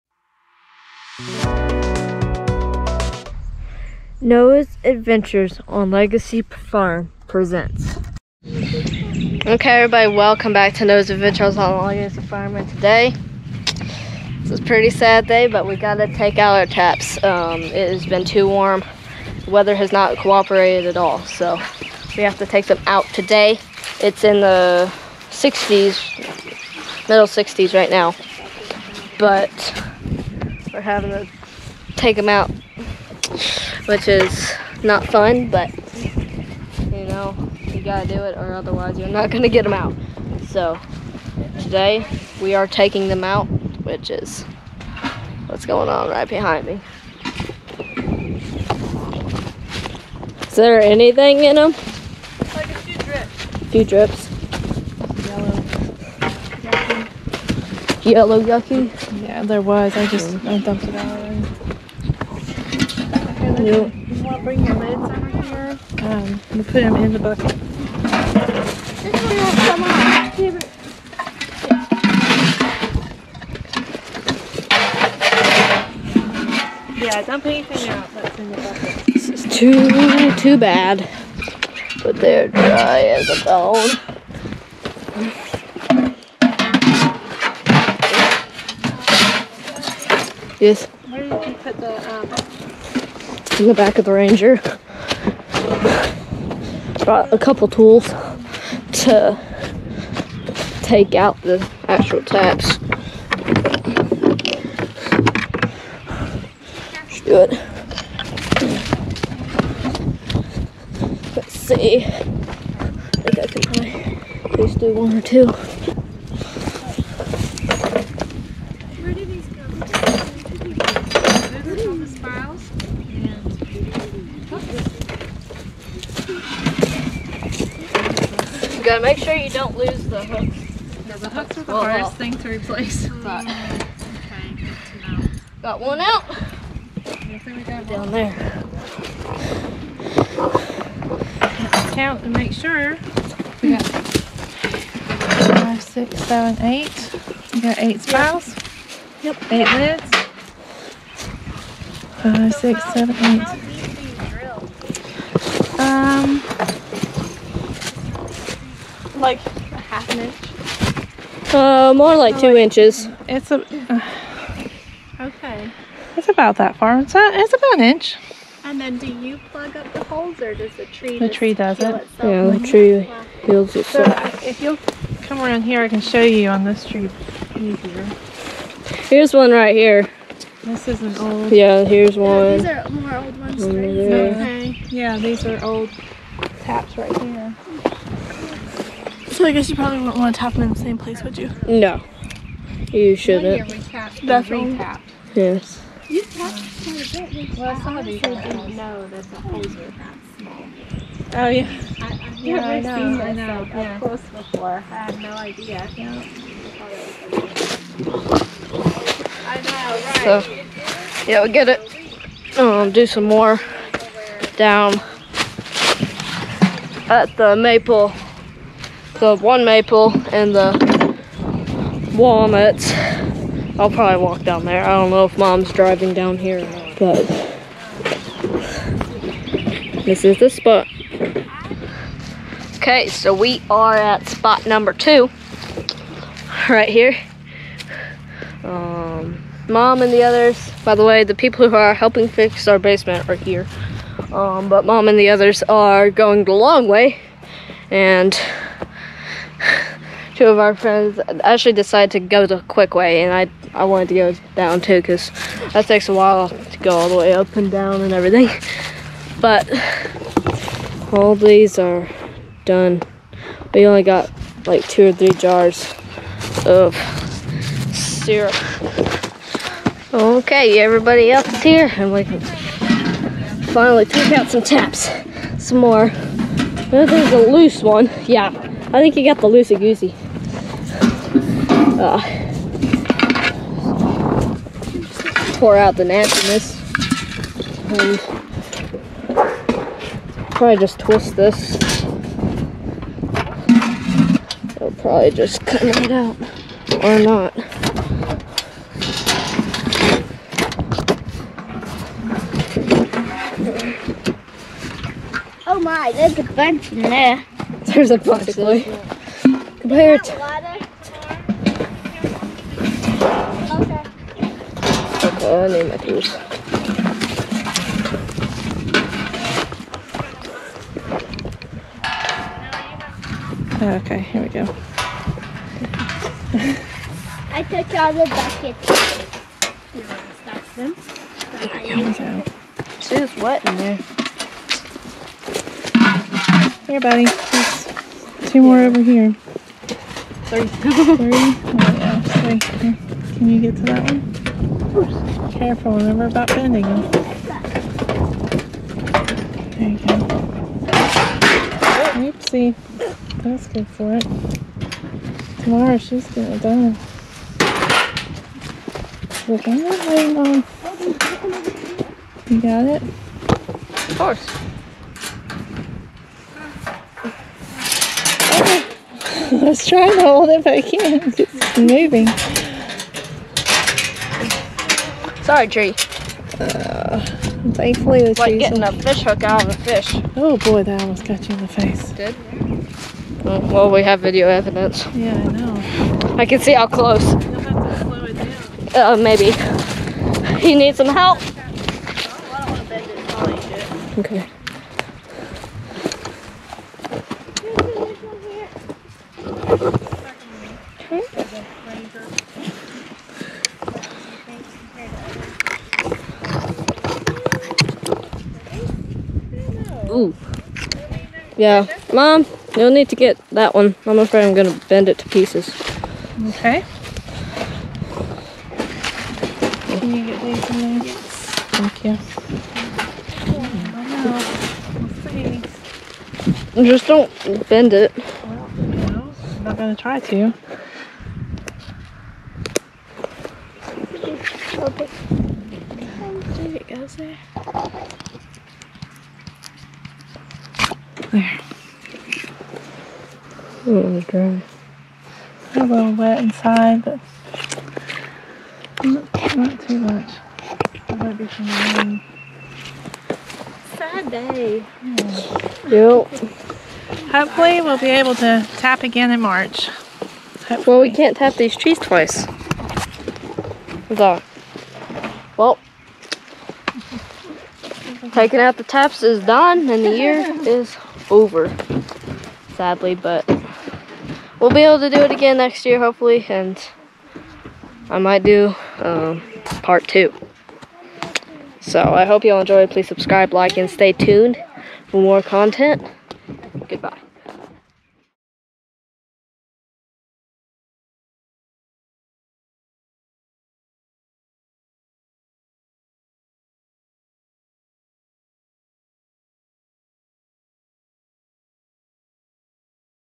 This Is ALWAYS A Sad Day!! Removing our spigots and buckets.| Part 3
This video shows how to remove your taps from a tree. This is always sad to see the taps being removed-which shows that the season is OVER!!! Especially this year because we got next to NOTHING in syrup!! But hopefully next year will be a good year!

Noah

Chapters:
0:00-0:47 Intro
0:47-3:51 Removing Maple Spouts
3:51-4:28 Intro To Spot #2
4:28-7:36 Removing Walnut Spouts
7:36-8:23 Outro